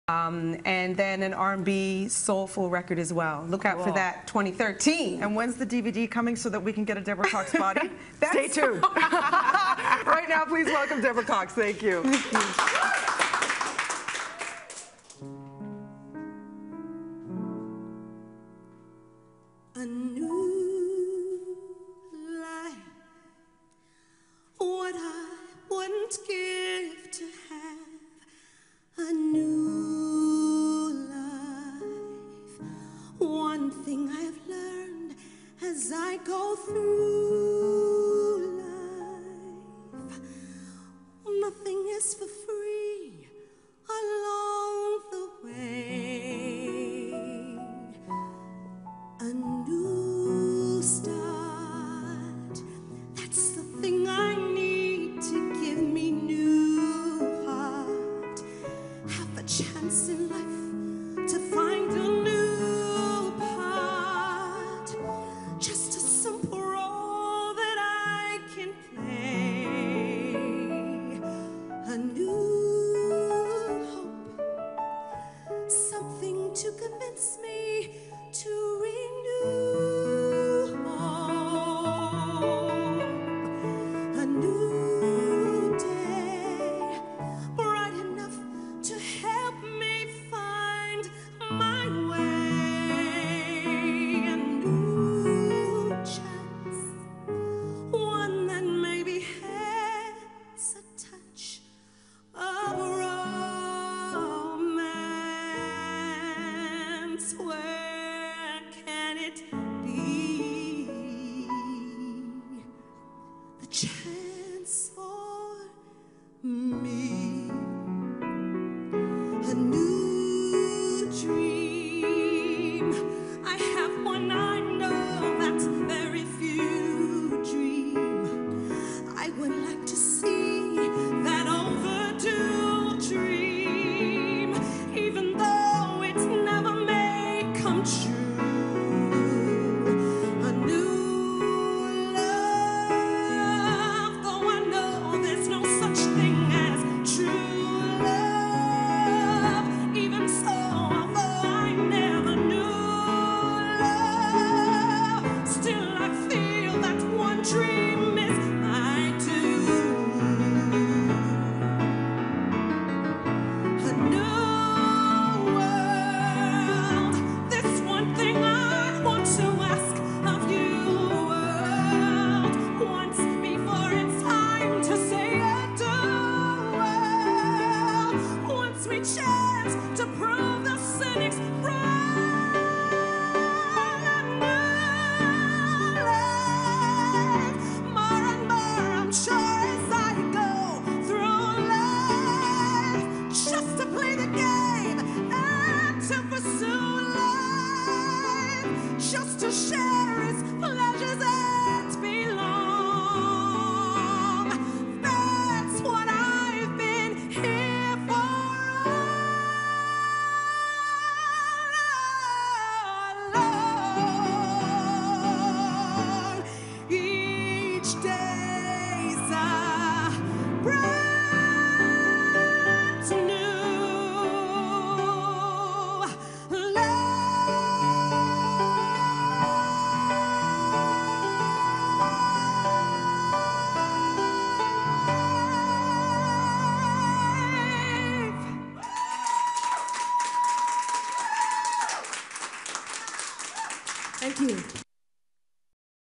Deborah Cox Performs Live
Deborah singing a selection from a popular musical.